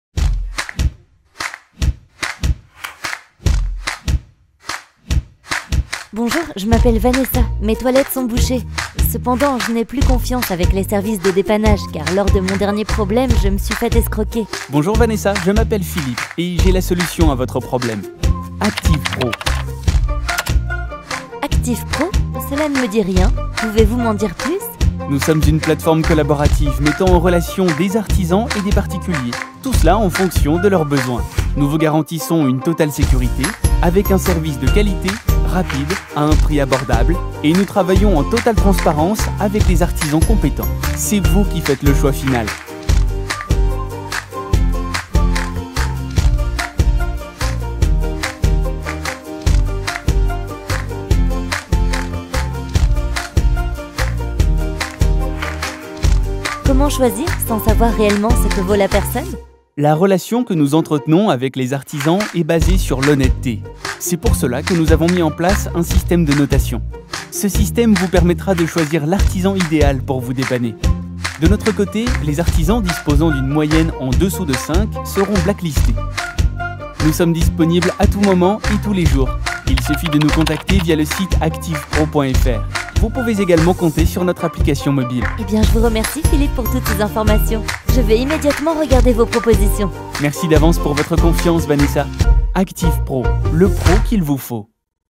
Promo Acteev Pro - Motion Design
Vidéo promotionnelle réalisée par la société VM-Prod pour le site collaboratif Acteev Pro

Description:
Trouvez leurs meilleurs artisans à proximité de chez vous, laissez votre avis, et rejoignez nous. La nouvelle plateforme collaborative qui vous assure un service de qualité que VOUS choisissez.

Réalisation: Adobe After Effect & Premiere Pro
Voix off: Nelly Rebibo & Willy Amann

©VM-Prod